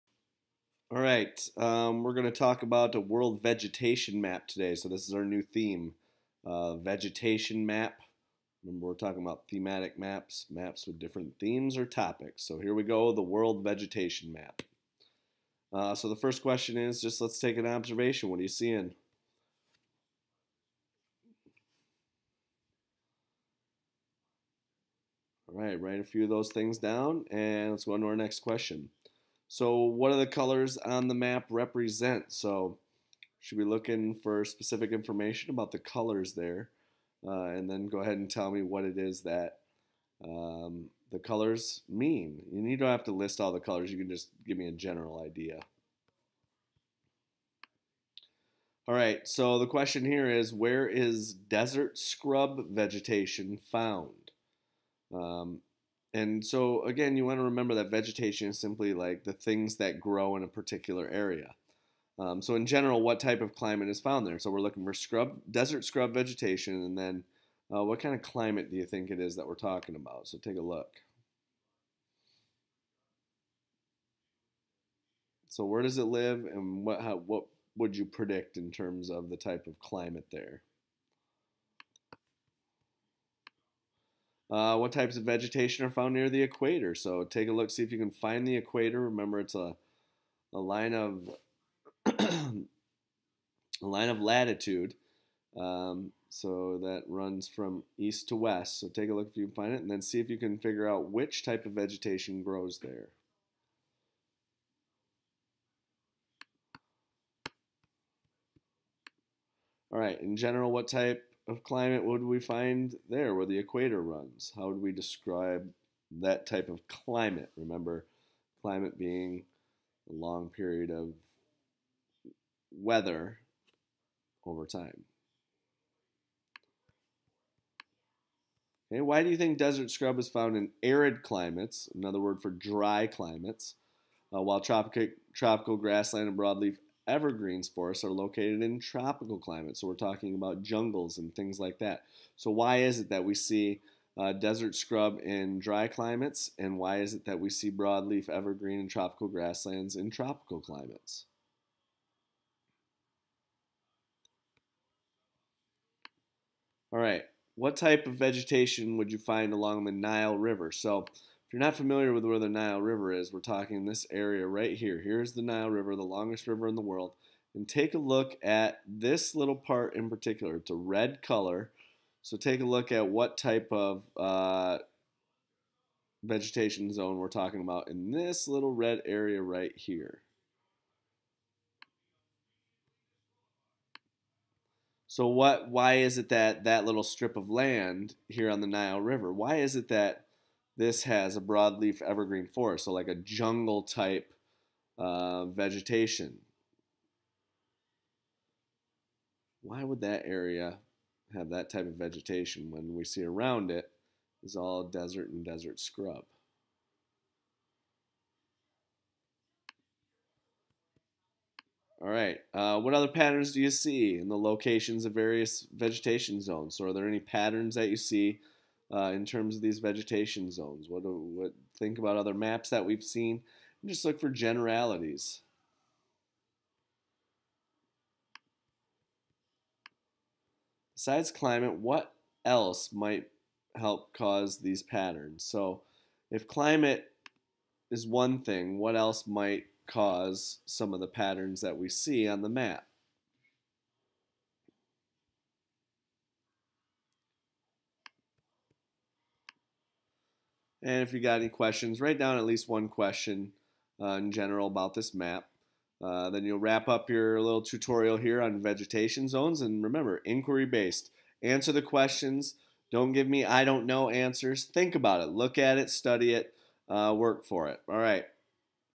Introduction to Vegetation Zones
Inquiry based introduction to global vegetation zones map. TCI Geography Alive!